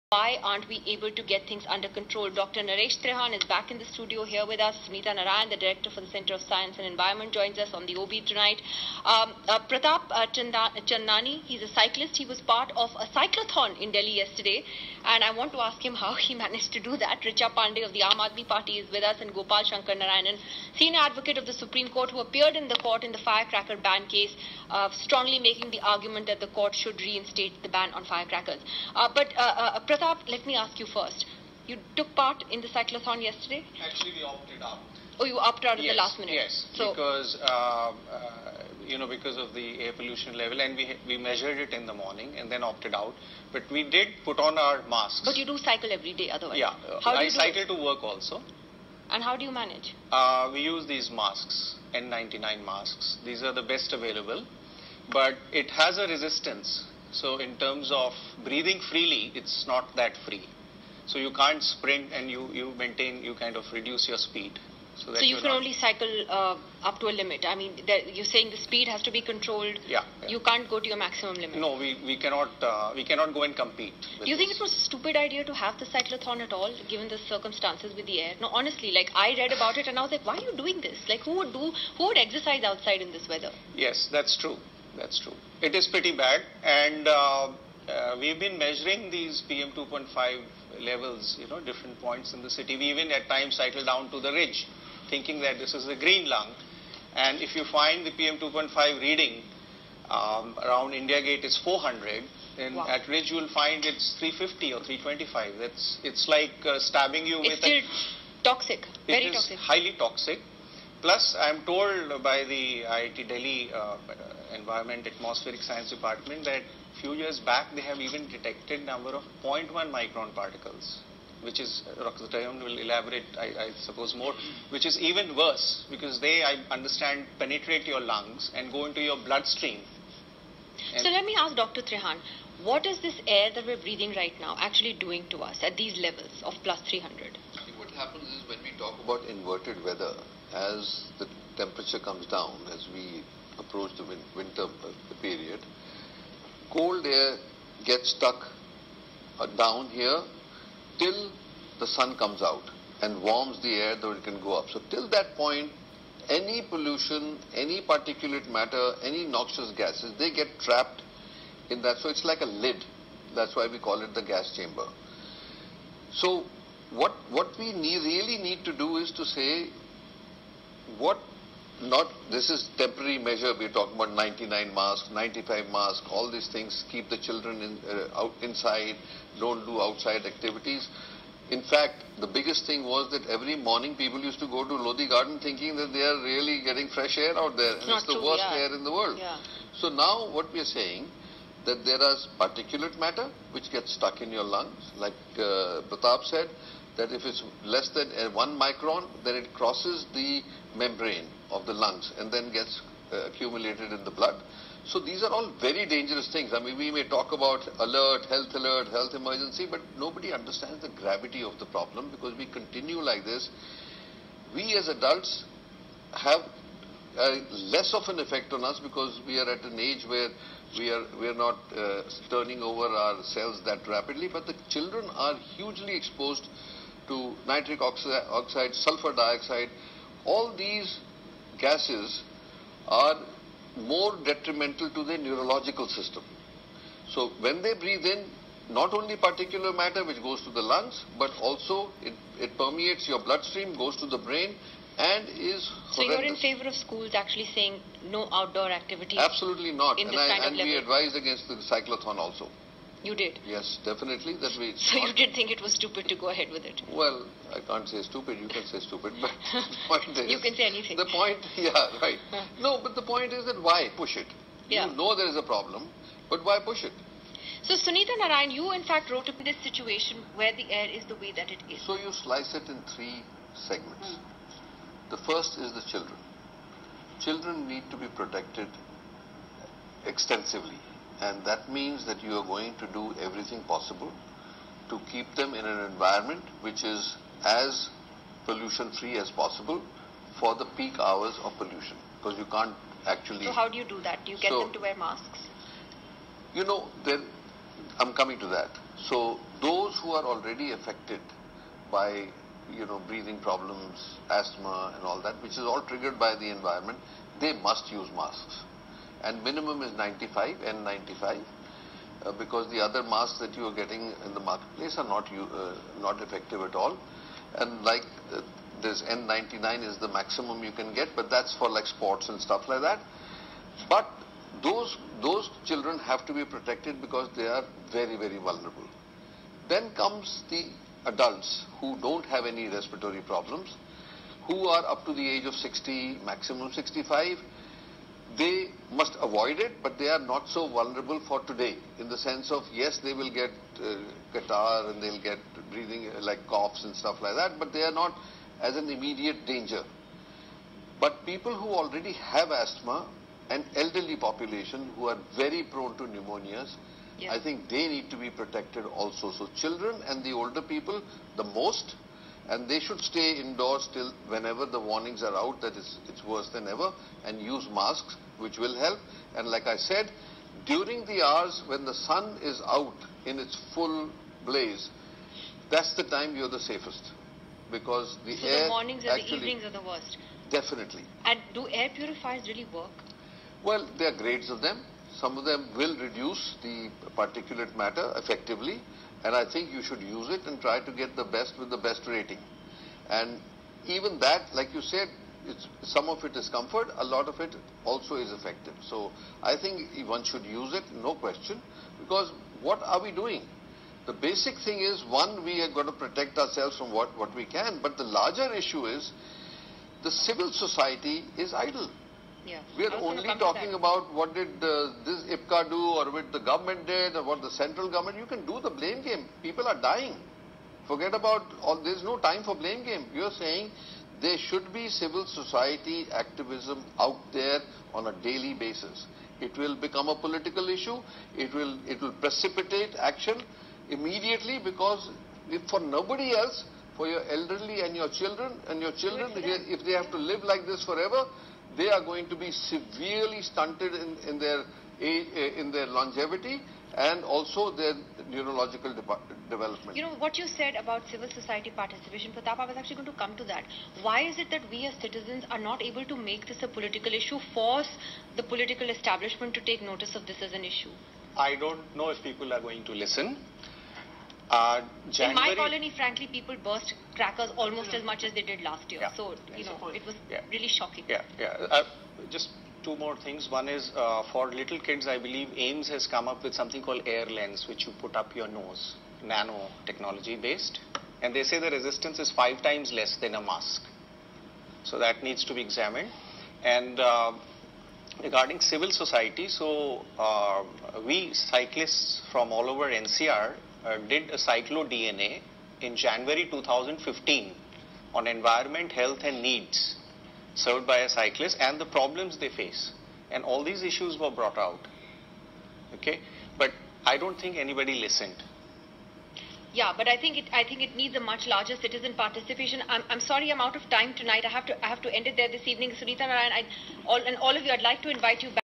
NDTV Show on Toxic Delhi Air with Dr Trehan & Others - Green Shakti | Remedies | Blue skies ?
On 06 November 2017, were invited by NDTV to participate in panel discussion on toxins in Delhi's air  and how citizens can adopt. atleast temporary measures, while long term solutions are worked out.